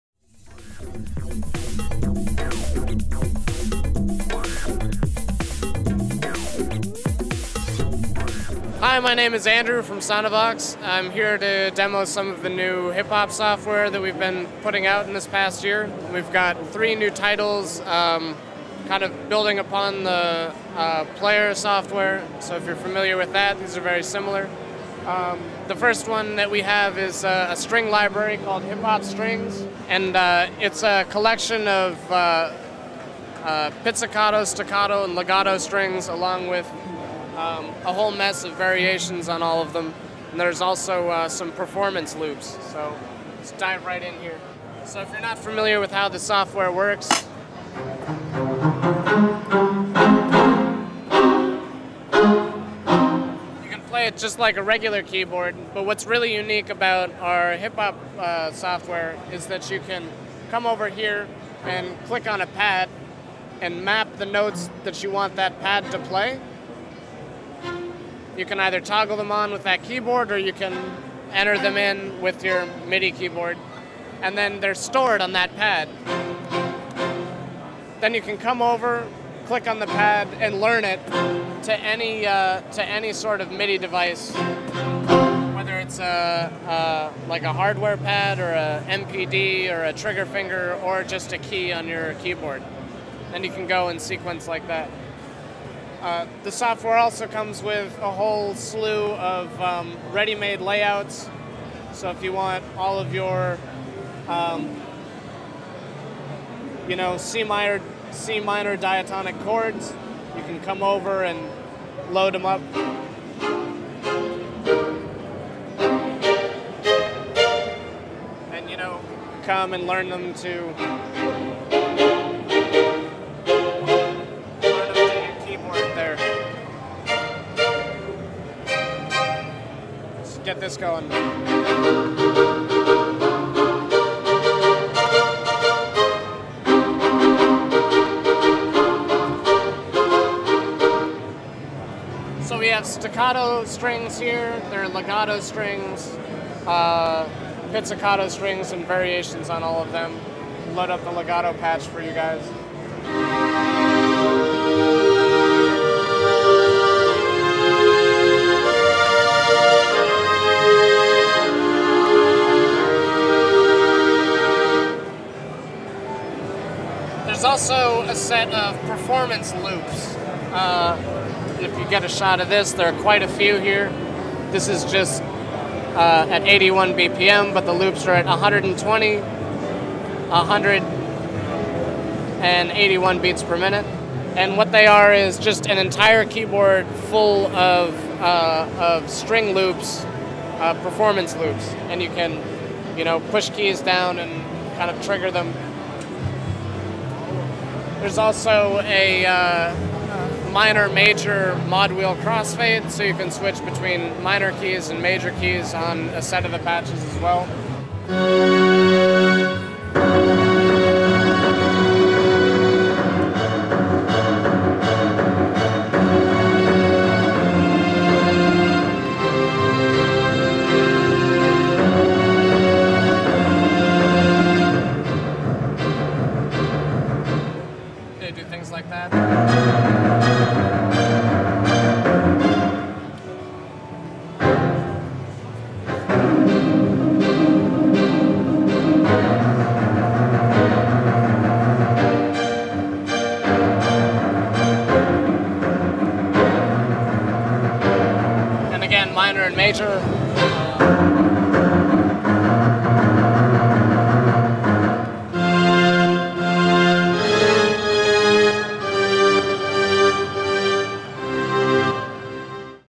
Sonivox Playa Hip-Hop Strings Blends Beats, Bows, And Bimproved Work Flow
Built from their smartly-designed Playa production interface, the new Sonivox Playa Hip-Hop Strings is the perfect way to throw down some Hot 97-ready violins, cellos and basses.*

To get an idea of just how easy the Playa interface makes hip-hop production -- and to get a listen to exactly what Hip-Hop Strings sound like -- we head over to the Sonivox booth at the recent NAMM. I dare you to watch this video demo and not dream about the day when some studio greenlights Notorious B.R.A.V.E.H.E.A.R.T.

*yeah I left out violas. Sorry, they're just not street enough.